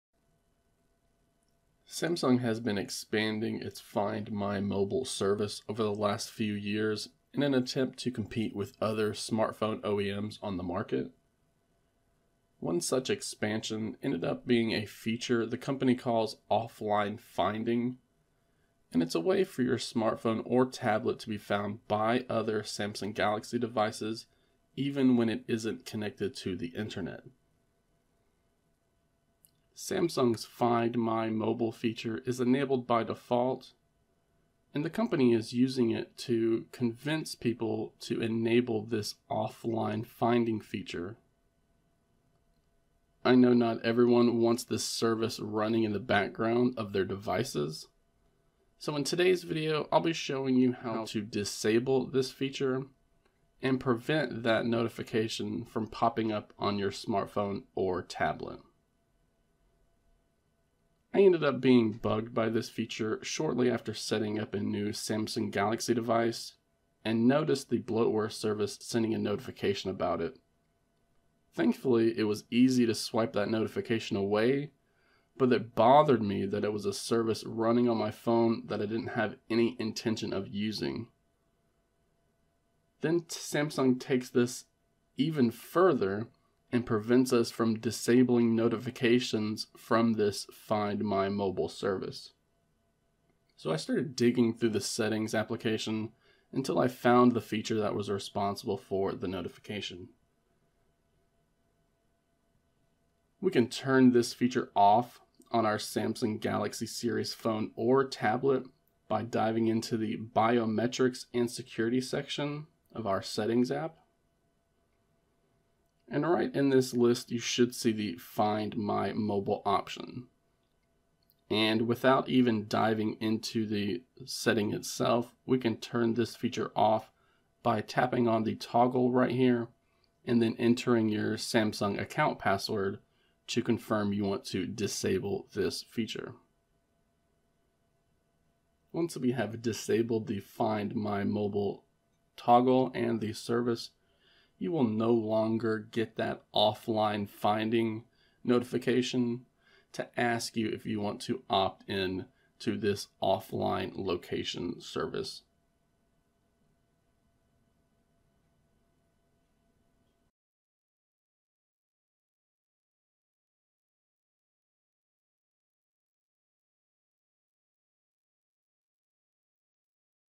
How to Disable the Find My Mobile Offline Finding Service & Notification on Samsung Galaxy Devices?
If your Samsung Galaxy smartphone or tablet has displayed an Offline Finding notification that you can't block, I can show you how to disable the feature entirely.

Video Description
Samsung has been expanding its Find My Mobile service over the last few years in an attempt to compete with other smartphone OEMs on the market. One such expansion ended up being a feature the company calls Offline Finding and it's a way for your smartphone or tablet to be found by other Samsung Galaxy devices even when it isn't connected to the internet.

The feature is enabled by default but it is a service that not everyone wants to have running in the background.

So in today's video I'll be showing you how to disable this feature and prevent the notification from popping up on your smartphone or tablet.

I ended up being bugged by this feature shortly after setting up a new Samsung Galaxy device and noticed the bloatware service sending me a notification about it. Thankfully it's easy to swipe the notification away but it bothered me that it was a service running on my phone that I didn't have any intention of using.

Then Samsung takes it even further and prevents us from disabling notifications from this service.

So I started digging through the Settings application until I found the feature that was responsible for the notification.

We can turn this feature off on our Samsung Galaxy series phone or tablet by diving into the Biometrics and Security section of the Settings app.

Now you should be able to see this Find My Mobile service right here in the list. Remember, this Find My Mobile feature is enabled by default so if you don't want it on your phone then you'll need to disable it from this toggle here.

1. Intro [00:00]
2. Open the Settings app [01:00]
3. Go into the "Biometrics & Security" section [01:50]
4. Disable the "Find My Mobile" toggle [02:19]